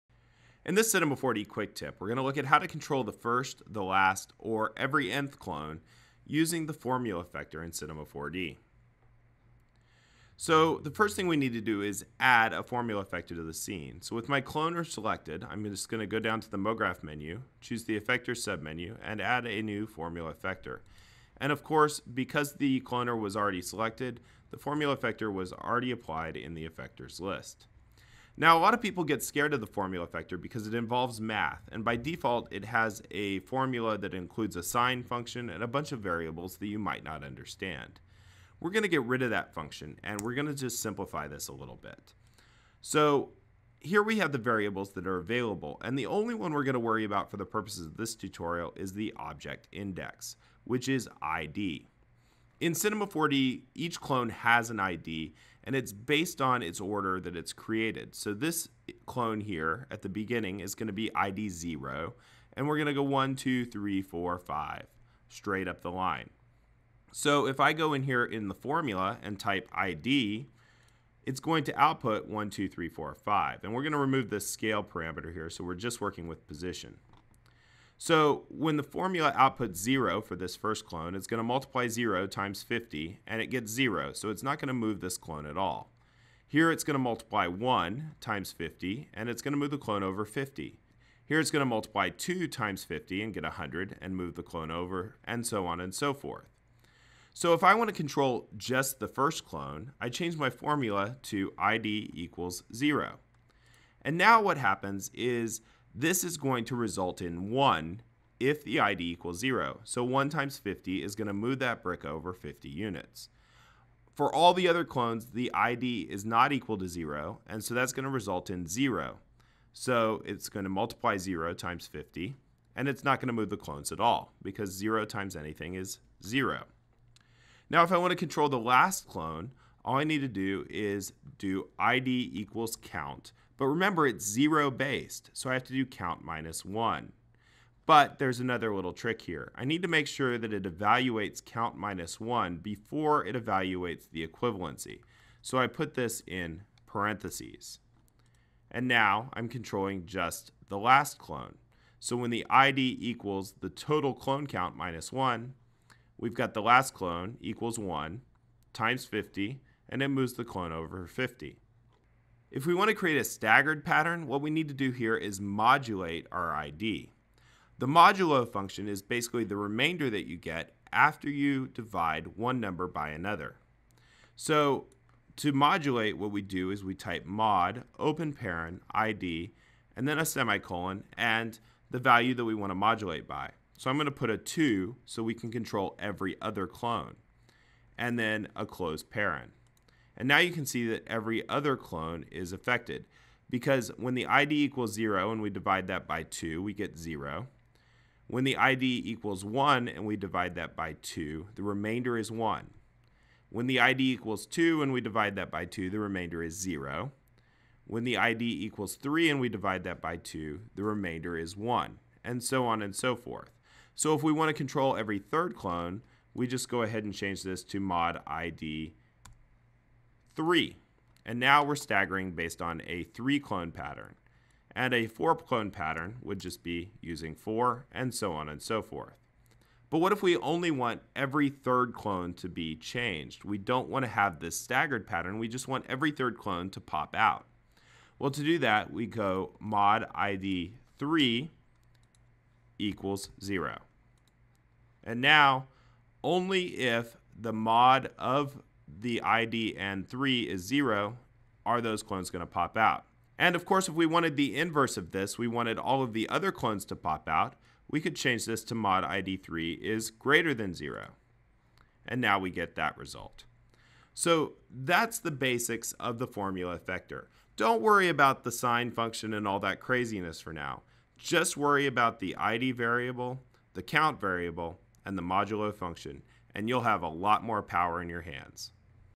Cinema 4D Quick Tip: Effect C4D Clones Based on ID With the Formula Effector
Learn the basics of the MoGraph Formula Effector in Cinema 4D, and how it can be used to easily transform the first or last clone using the ID and count variables, create stepped patterns using modulo, and adjust every second, third, fourth, or nth clone.